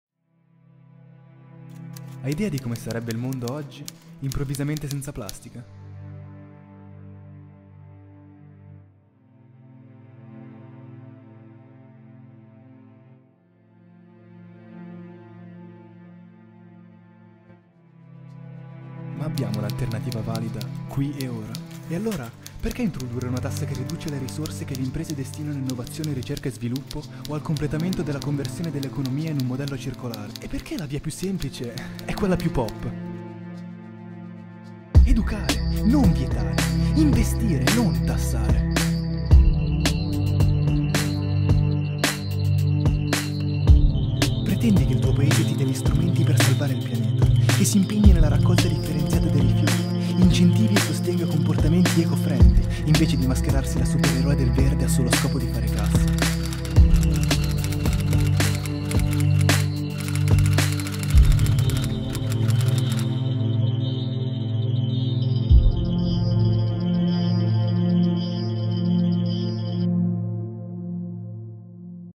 Simec Group
Ci sono occasioni in cui schierarsi e far sentire la propria voce è un dovere: Simec Group sulla Plastic Tax.